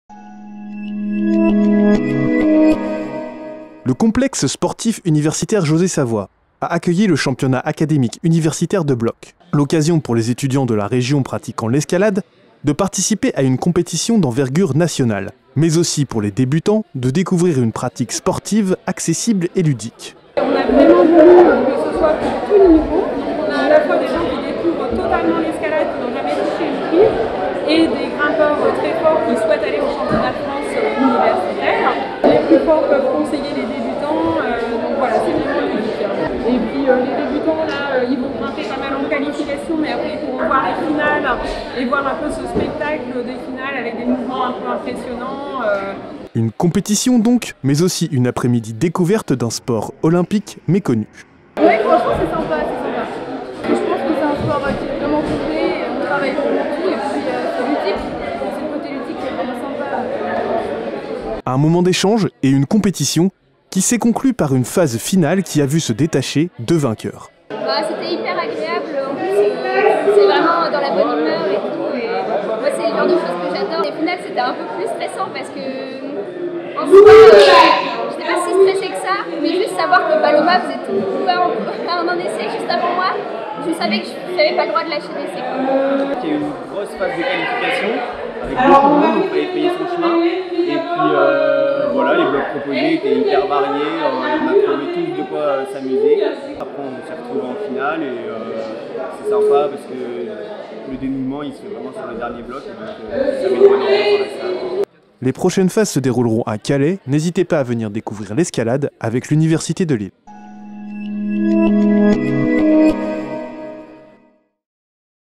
Championnat académique universitaire de bloc
L'épreuve de bloc du championnat académique universitaire d’escalade aura lieu sur le mur d’escalade de l’université de Lille, le jeudi 10 mars
Mise à jour le 08/03/2022 (13:17)

Ce championnat régional d’escalade 2022 se décompose en 3 épreuves :

    le bloc (sans corde, à une hauteur maximale de 4m50),
    la difficulté (voies en têtes)
    et la vitesse (voies en moulinette).
    Les épreuves de difficulté et de vitesse auront lieu sur Calais le 7 avril. Le classement à ces 3 épreuves permet de sélectionner les quelques grimpeurs/euses qui iront au championnat de France universitaire.

Qualifications et finales
Les 6 meilleurs blocs enchaînés (atteinte de la dernière prise avec les 2 mains) pendant la phase de qualification permettront de générer le classement et de sélectionner 5 grimpeurs et 5 grimpeuses pour les finales. Les 3 blocs de finales hommes et 3 blocs de finales femmes seront l’occasion, pour les spectateurs, d’admirer des techniques de grimpe variées, des mouvements parfois spectaculaires et de vivre le ‘suspense’ et les rebondissements suscités par la lecture et la progression des grimpeurs/euses pour atteindre le TOP (le haut du bloc).

Nous vous attendons nombreux :

    pour les étudiants, en tant que participants, pour simplement découvrir l’escalade ou bien pour essayer de vous qualifier au championnat de France en vous inscrivant sur :

sport.univ-lille.fr